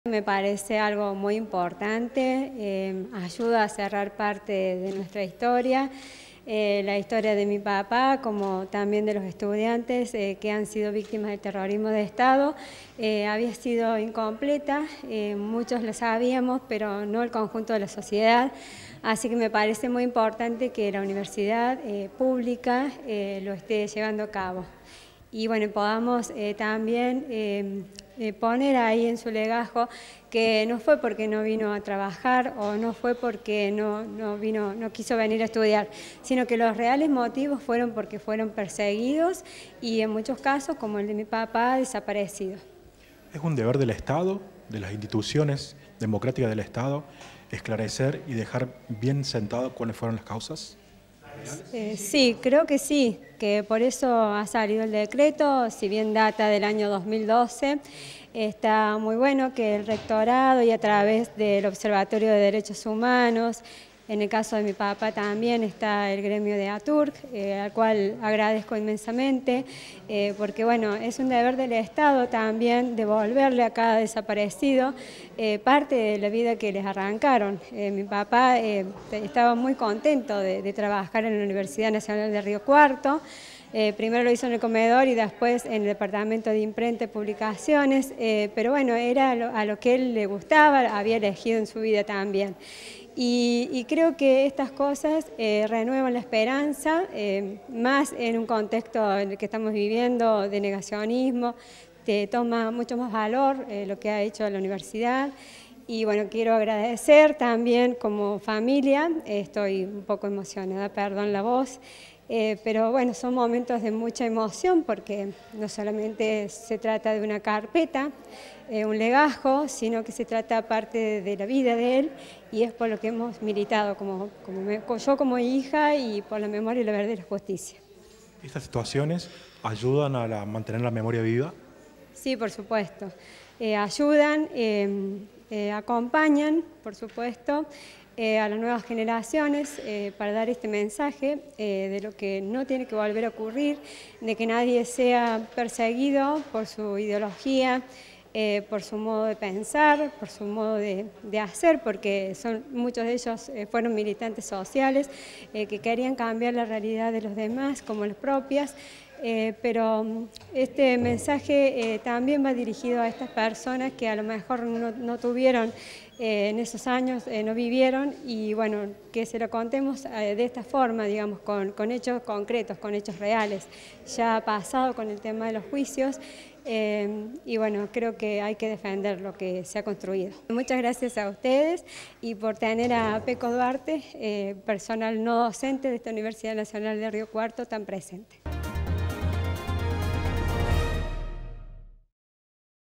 PRIMER ACTO DE REPARACIÓN DE LEGAJOS DE TRABAJADORES Y ESTUDIANTES VÍCTIMAS DE LA DICTADURA MILITAR
CLARISA DUARTE – HIJA DE JOSÉ ALFREDO “PECO” DUARTE
UniRio TV
Universidad Nacional de Río Cuarto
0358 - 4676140